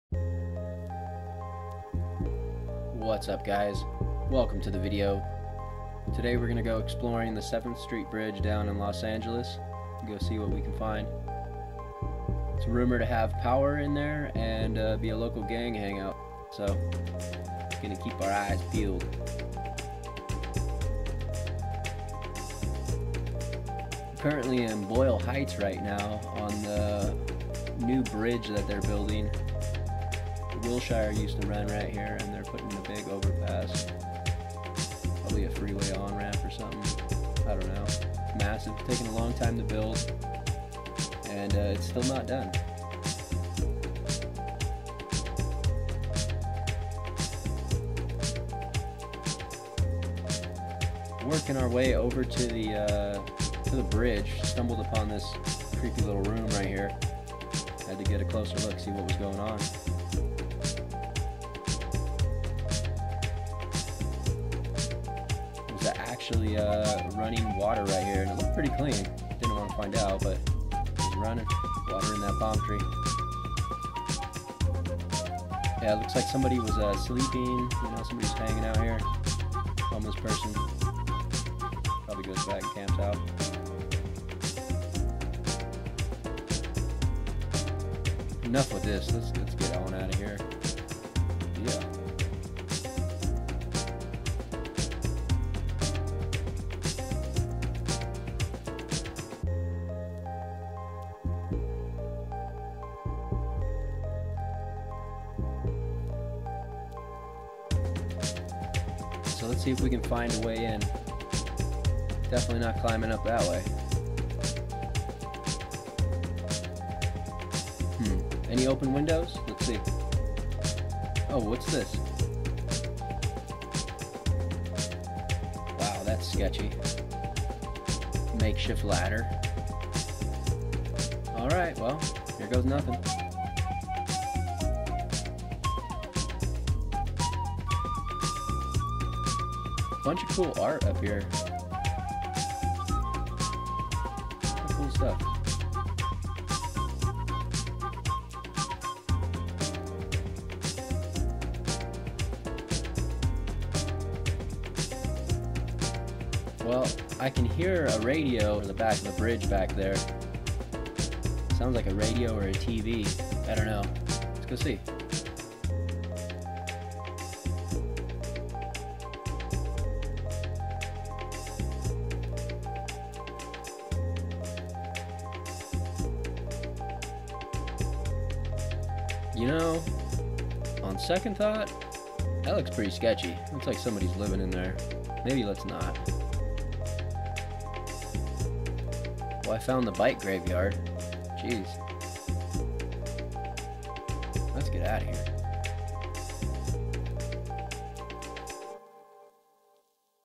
7th Street Bridge Adventure (Found Water & Power)
Finding Locating and accessing the 7th street bridge was definitely an adventure. The journey starts in Boyle Heights. I found clean running water an power in the bridge. If you have any questions or concerns leave it in the comments thanks and peace out.